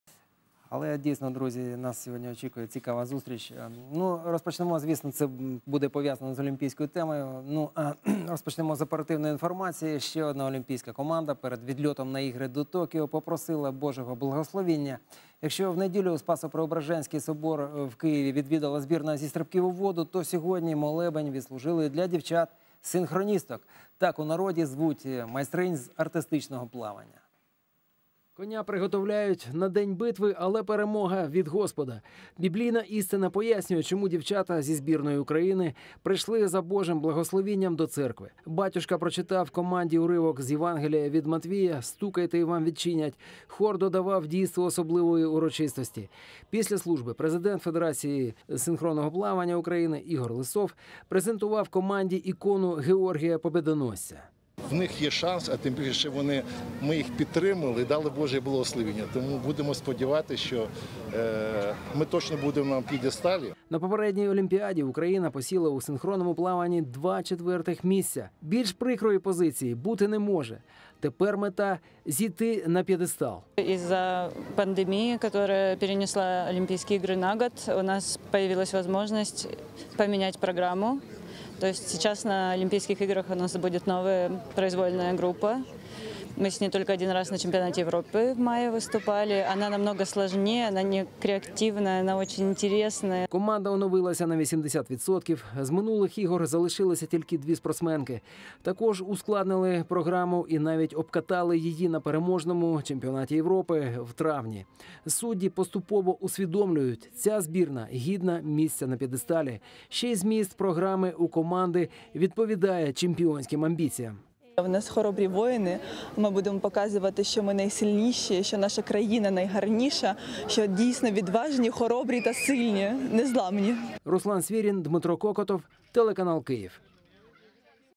Артистичне плавання: Благословіння перед стартом
Збірна України з артистичного (синхронного) плавання перед відльотом до олімпійського Токіо відвідала Спасопреображенський храм у Києві і попросила Божого благословіння. @sribnok скористався нагодою побачити команду чарівних плавчинь.